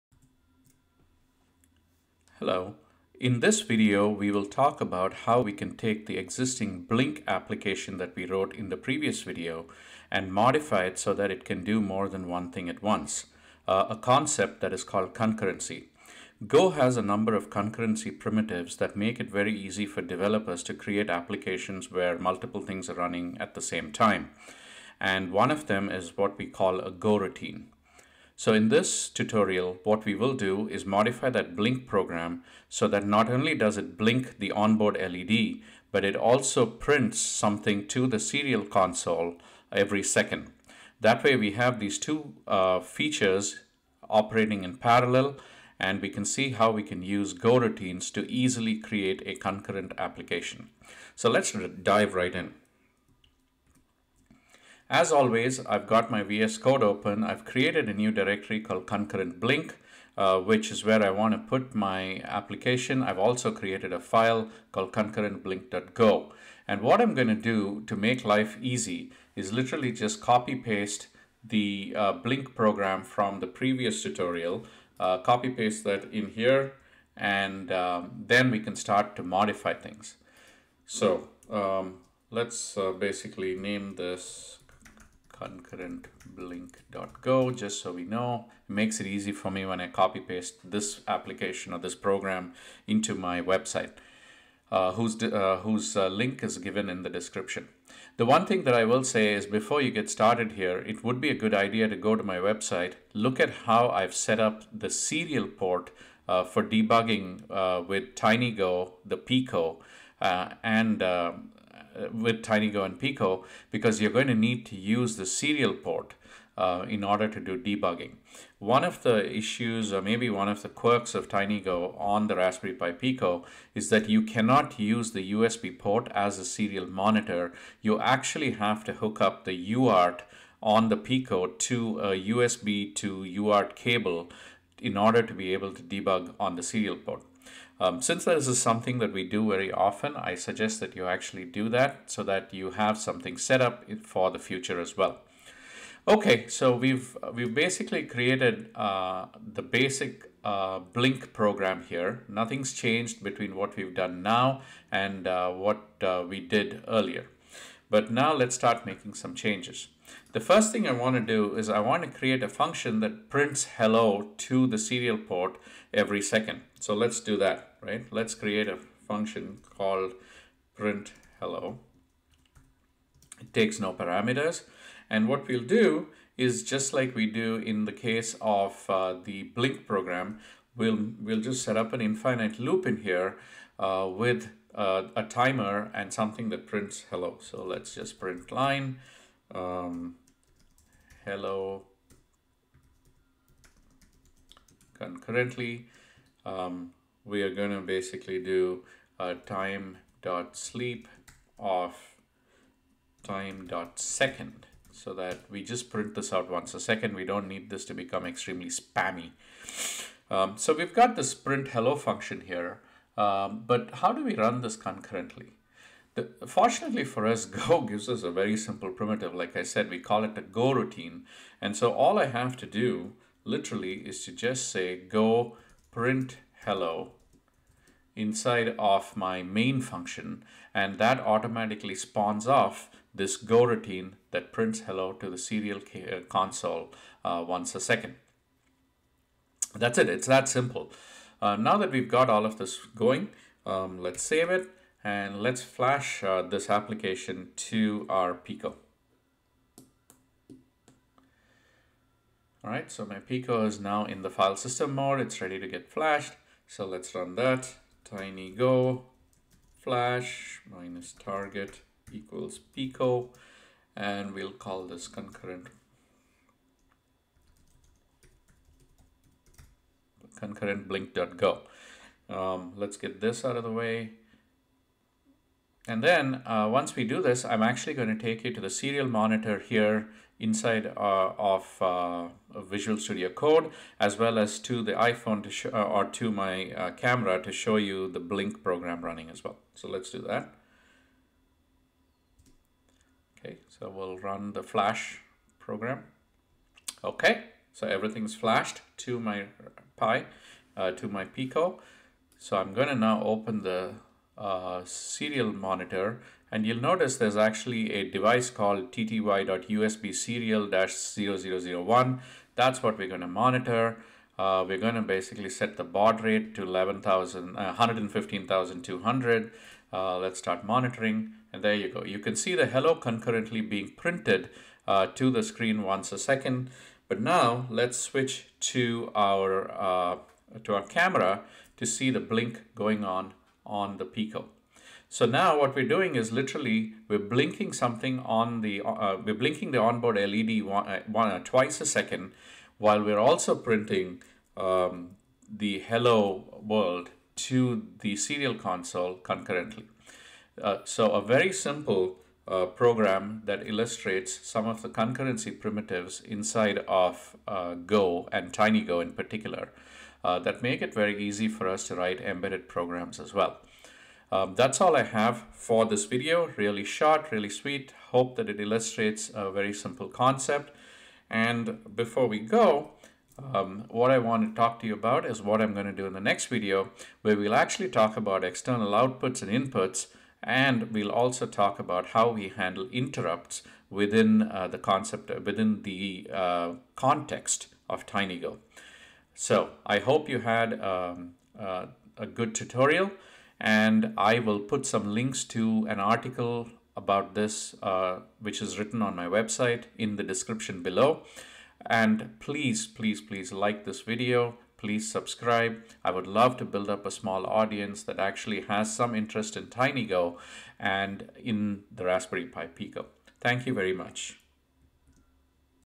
TinyGo and Raspberry Pi Pico: Concurrency (GoRoutines)
Today, I walk you through a simple program that illustrates how one can use GoRoutines to easily create a program that runs two concurrent functions.

GoRoutines greatly simplify the code required to create concurrent functions and they are extremely lightweight.